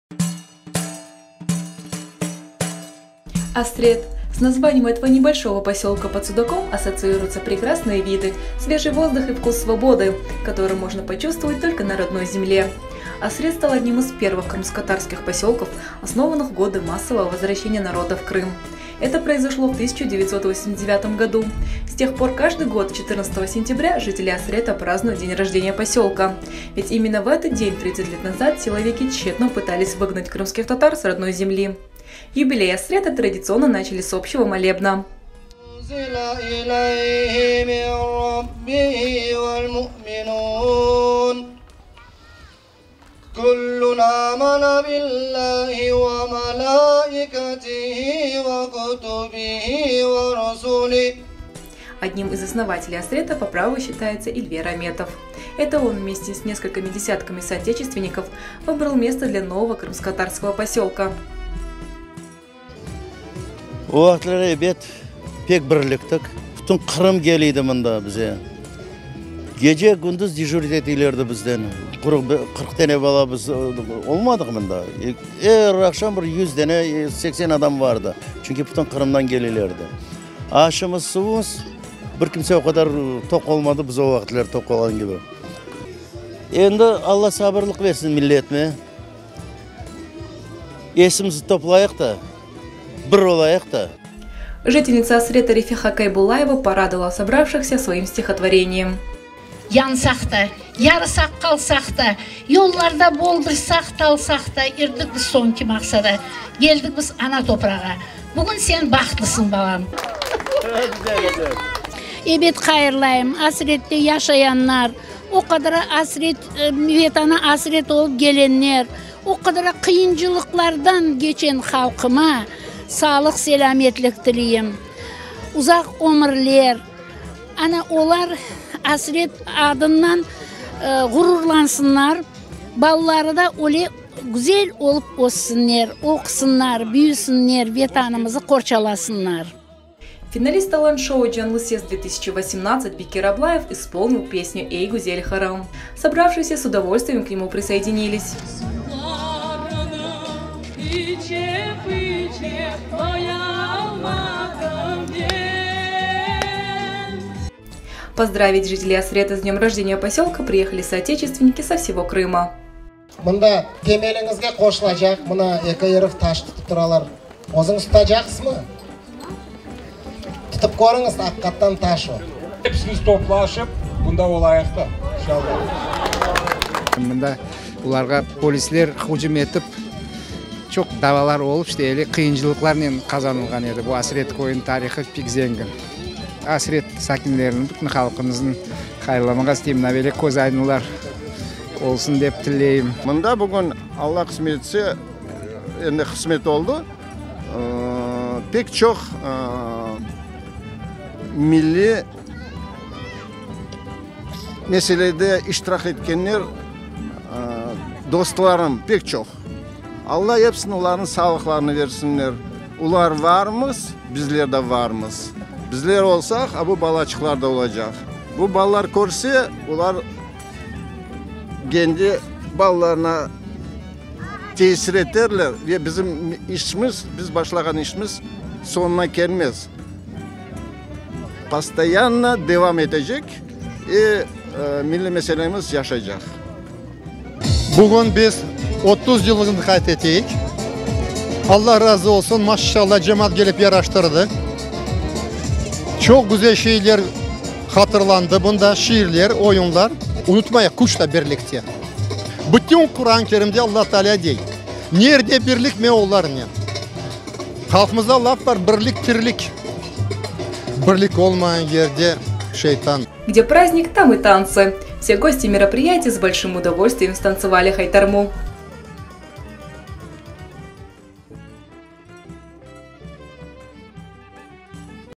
Крымскотатарский посёлок Асрет отметил своё 30-летие
Одним из первых крымскотатарских посёлков, основанных в годы массового возвращения народа на Родину, стал расположенный под Судаком Асрет. В минувшие выходные он отметил своё 30-летие.